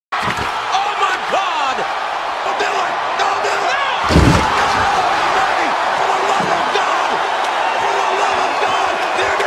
When Wrestling is LIFE!!
late night wrestling with your siblings? You have to share this with a friend! FB.Com/gamethirty